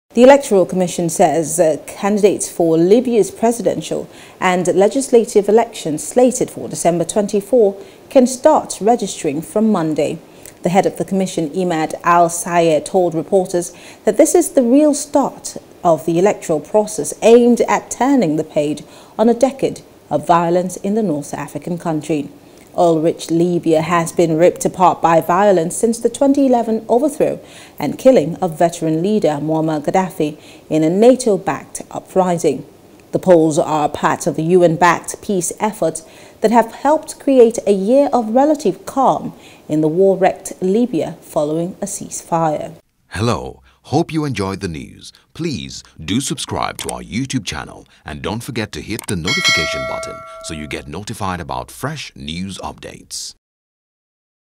Electoral Commission To Open Registration For Presidential And Legislative Polls | AFRICAN
The electoral commission says candidates for Libya's presidential and legislative elections slated for December 24 can start registering from Monday